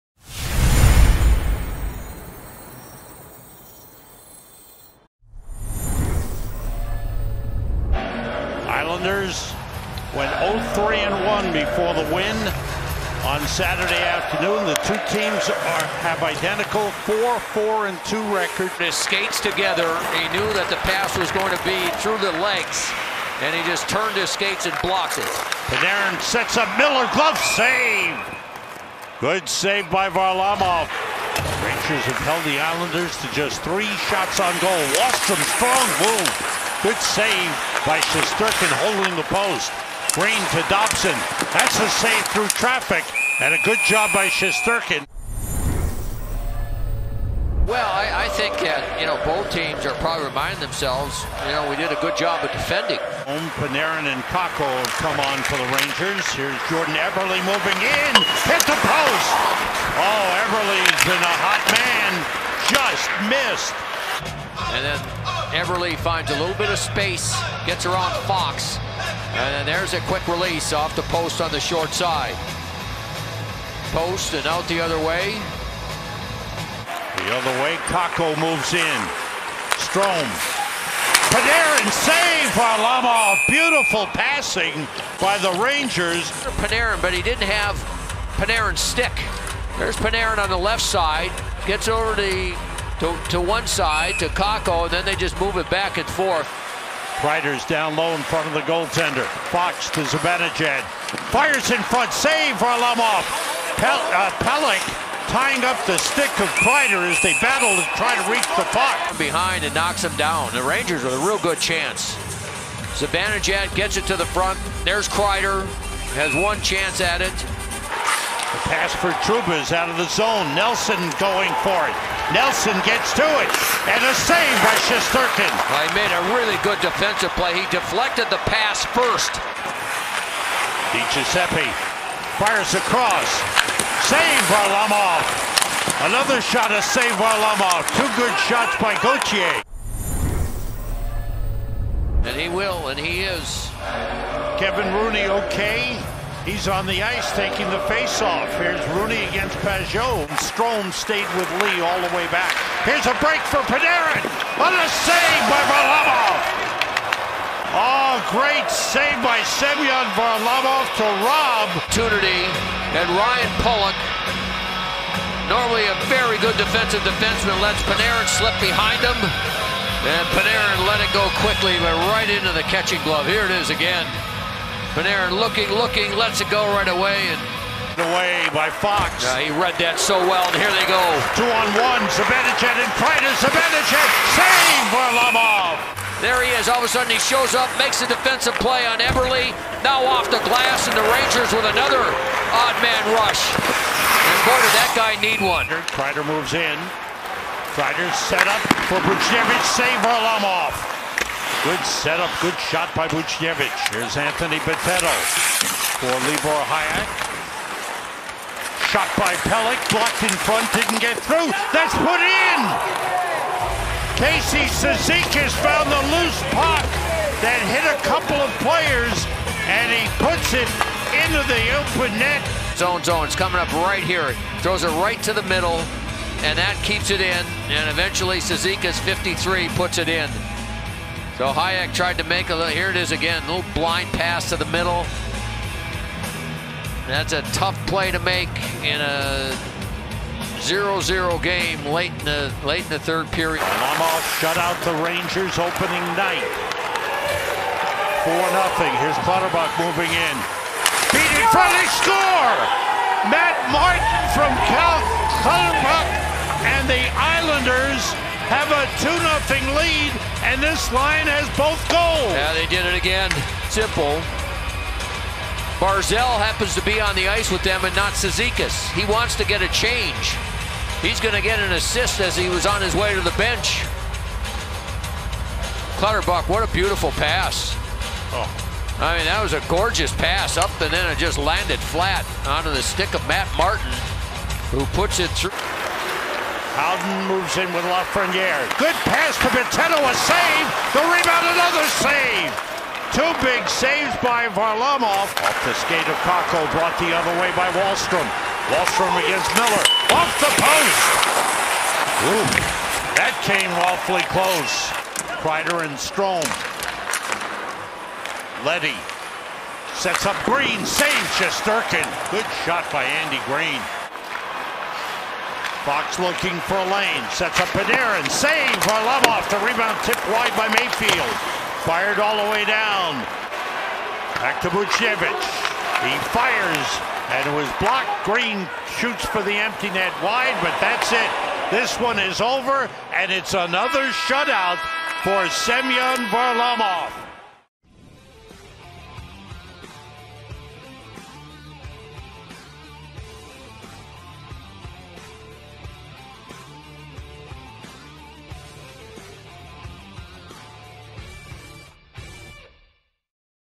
NHL Game Highlights | Islanders vs. Rangers - Feb. 8, 2021
Semyon Varlamov made 30 saves for the shutout as the New York Islanders beat the New York Rangers 2-0.